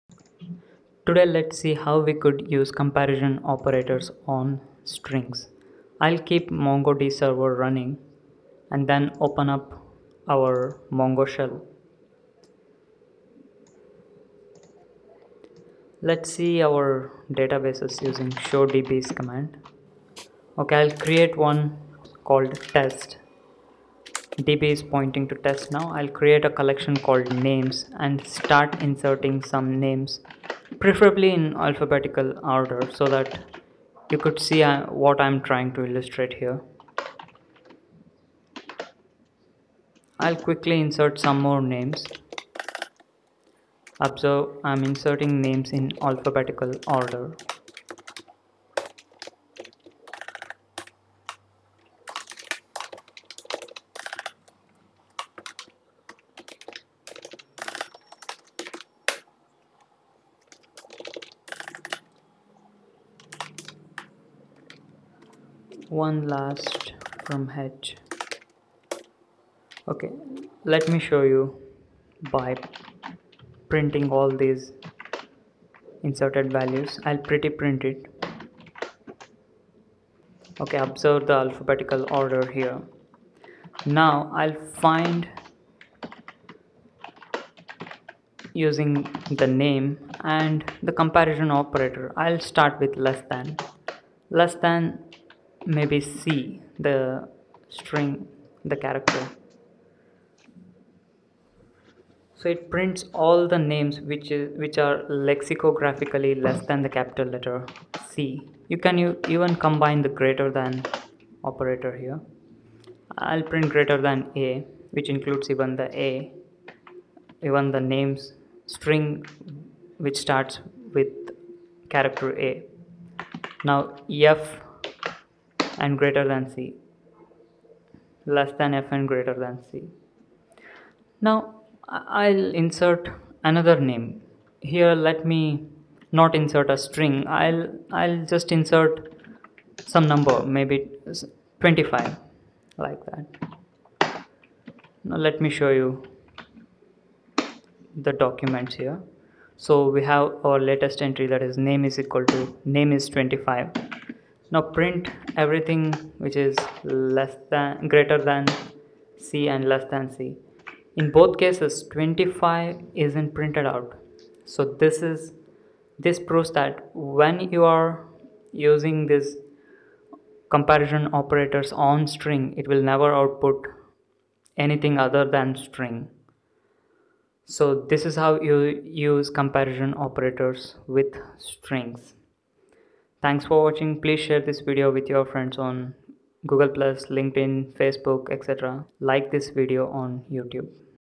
Comparison Operators on Strings: MongoDB
Today we shall see how we can compare strings using comparison operators like,
$ne -- not equal to
$gt -- greater than
$gte -- greater than or equal to
$lt -- less than
$lte -- less than or equal to